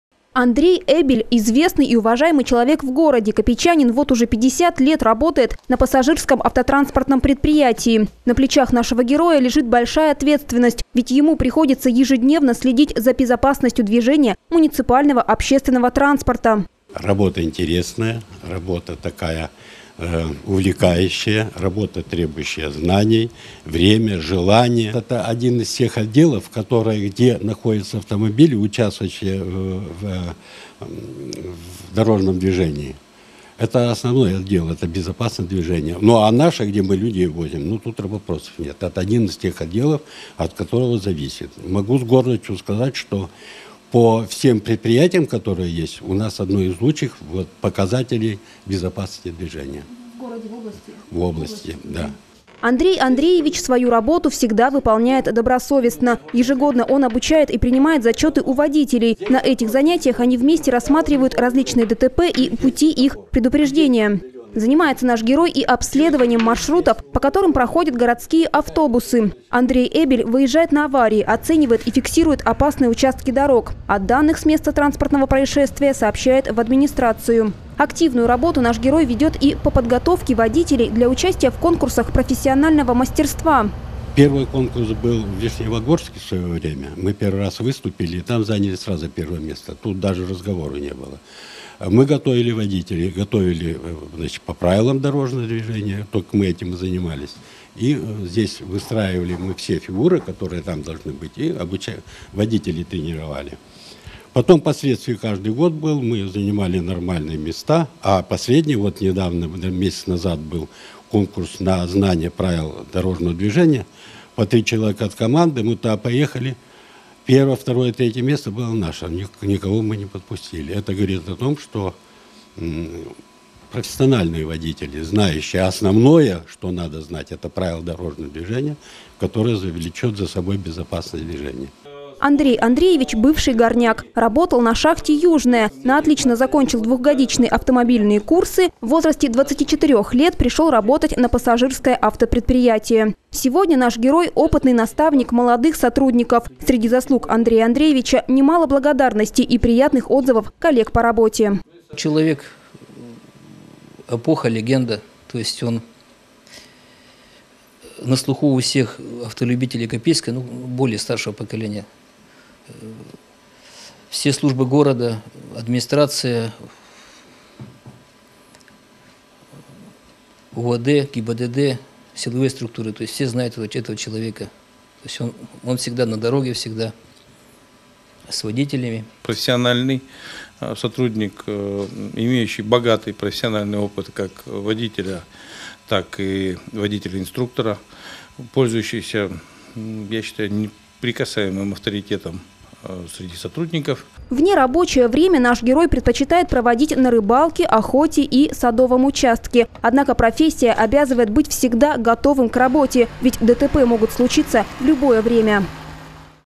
Водитель-инструктор Андрей Эбель в рубрике «Один из народа»
Андрей Эбель – известный и уважаемый человек в городе. Копейчанин вот уже 50 лет работает на пассажирском автотранспортном предприятии. С Андреем Андреевичем знакомим вас в рубрике «Один из народа».